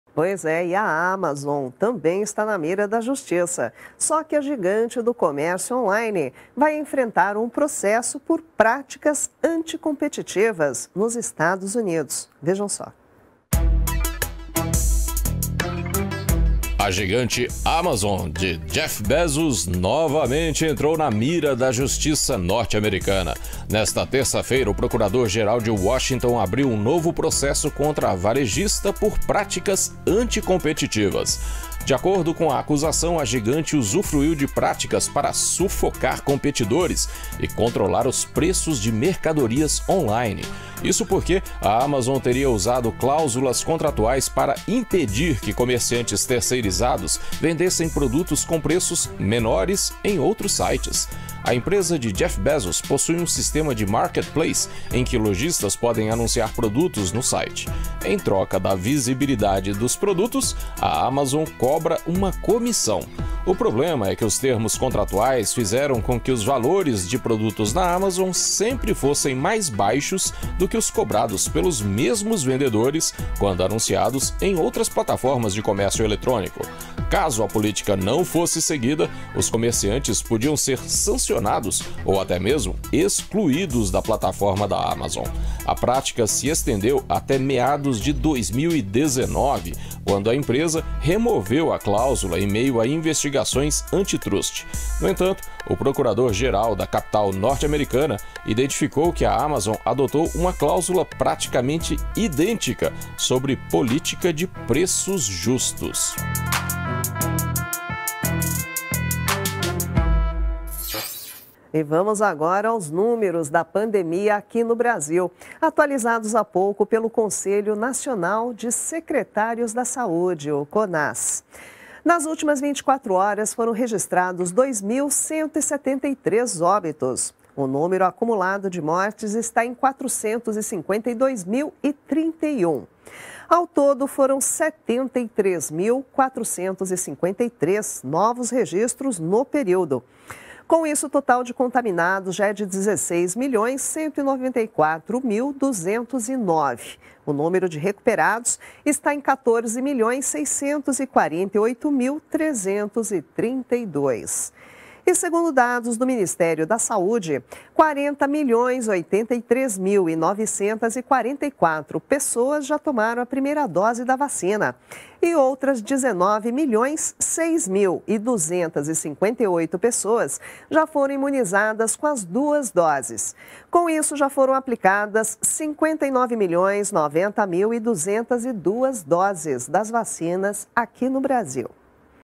Amazon é processada por práticas anticompetitivas nos EUA
A gigante Amazon, de Jeff Bezos, novamente entrou na mira da Justiça dos Estados Unidos. A empresa é acusada de pressionar vendedores para controlar preços de mercadorias online.